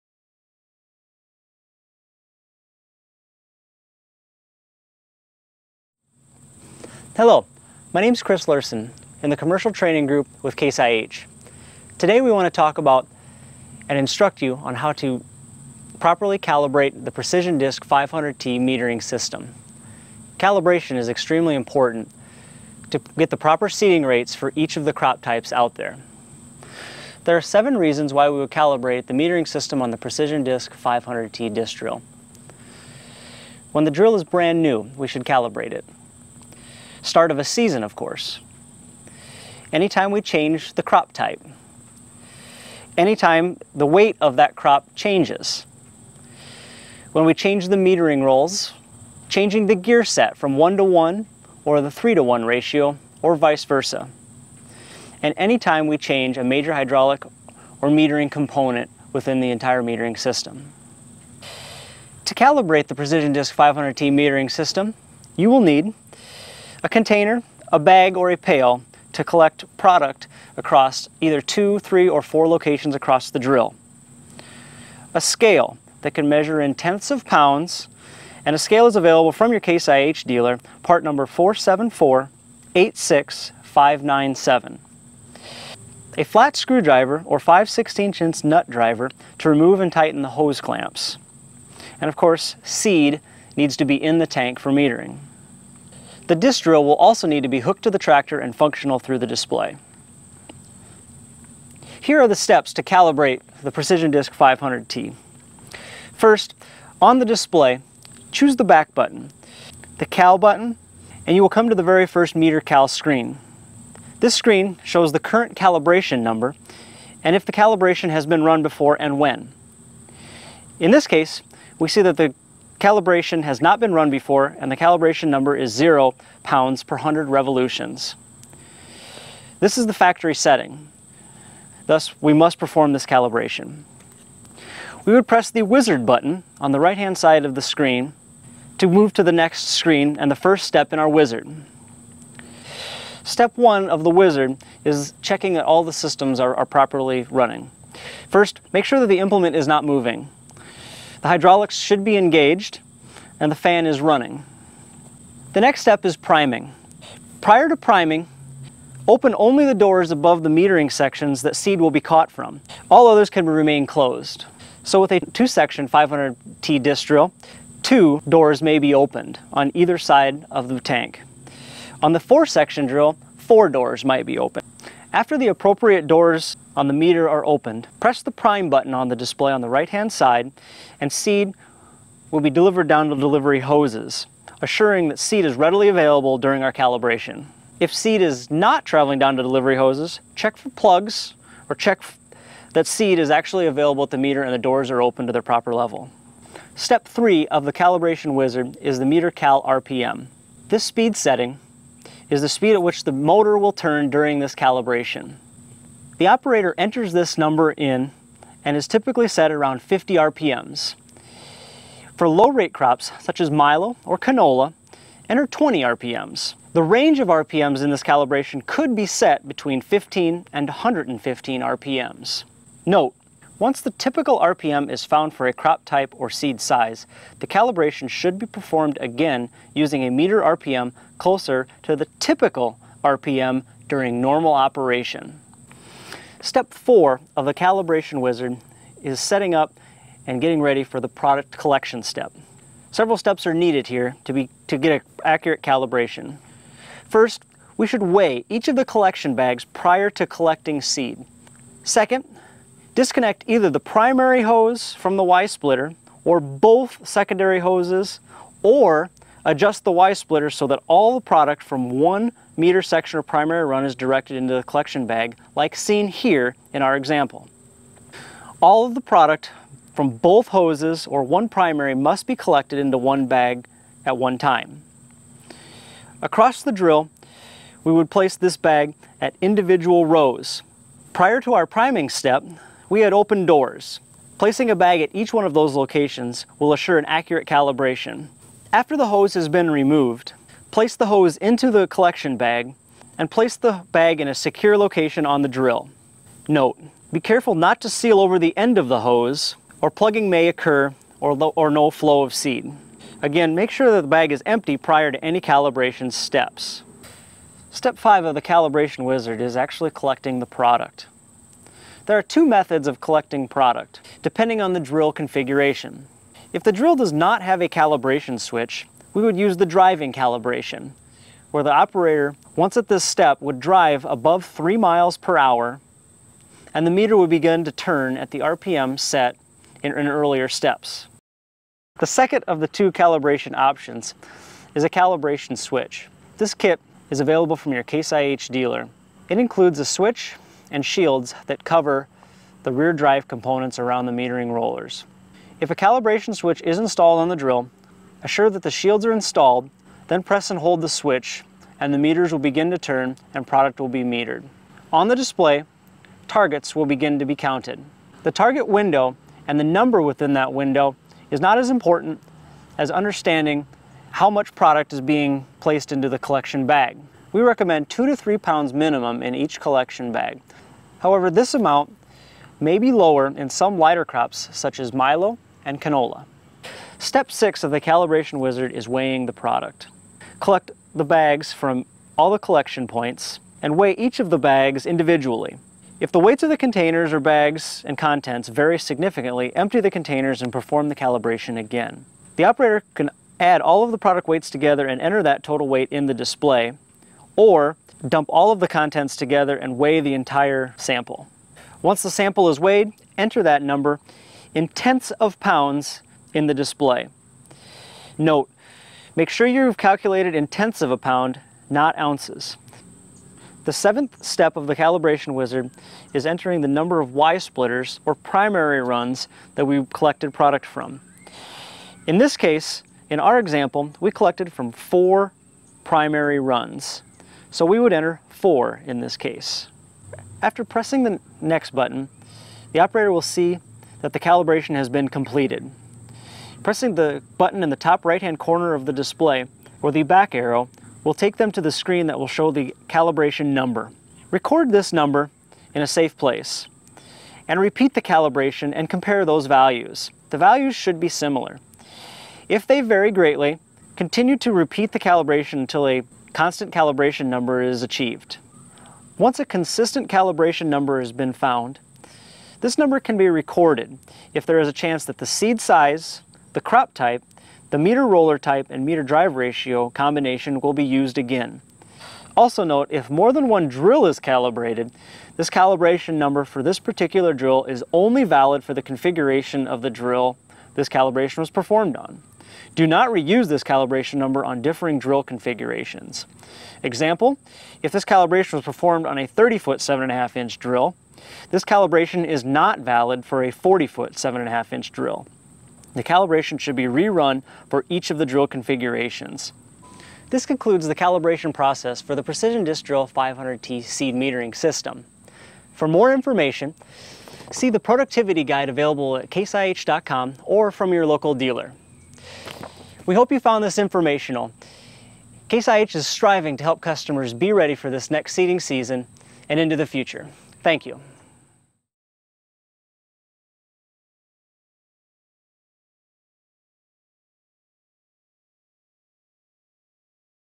Precision Disk 500T Calibration Tips
Calibration of the Case IH Precision Disk 500T is extremely important. Watch the video for helpful tips on how to calibrate yours.